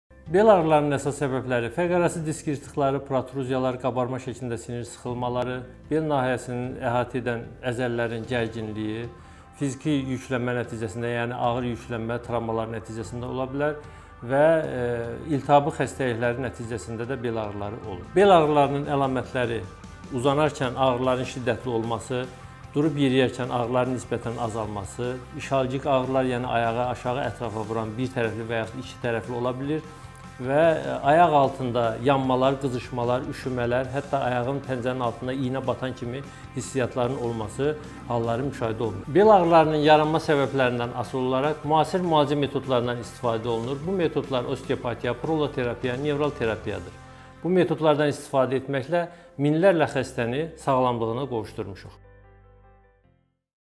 Bel ağrılarının əməliyyatsız müalicəsi
Revmatoloq , Osteopat Əli Əlizadə